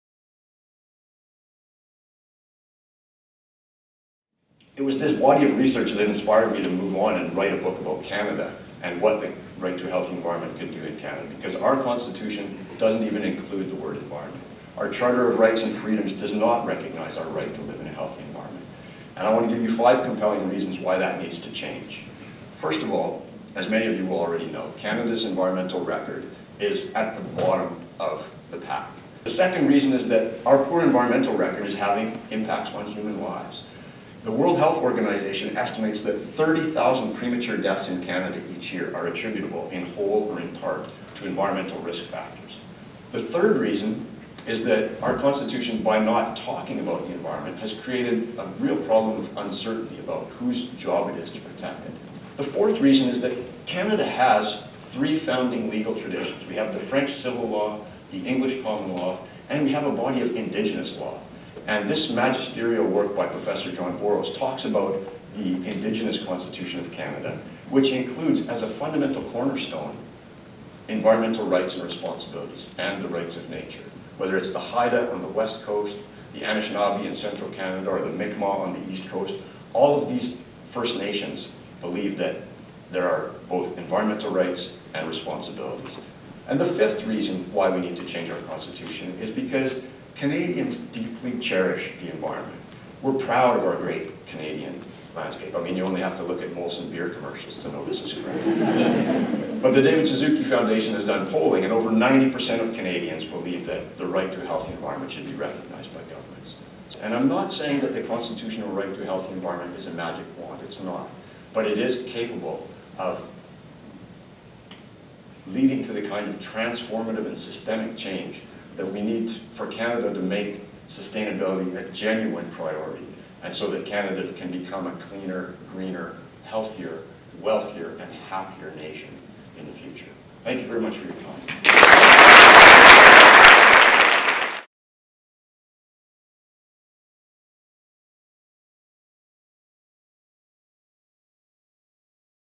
Five reasons Canadians need a constitutional right to a healthy environment
On November 27th, the David Suzuki Foundation and Ecojustice.ca presented a free lecture by environmental activist and lawyer, David Boyd, on his new book: The Right to A Healthy Environment". Over 400 people attended and participated in the question and answer period afterward.